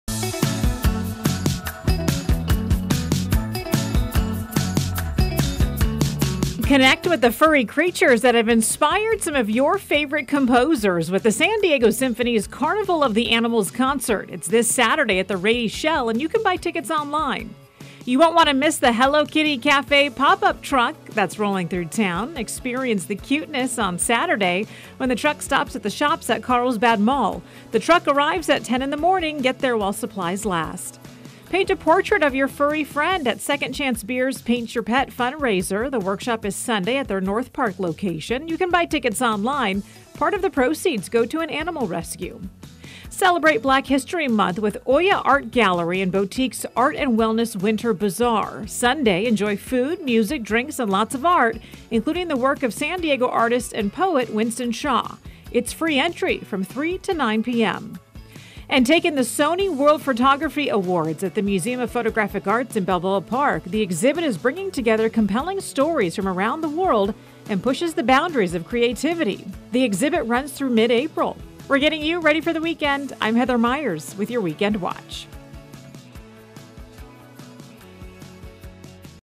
Weekend Watch February 17 - 19 | Things to do in San Diego
As always, there is so much to do this weekend in San Diego! Here are a few things you won't want to miss.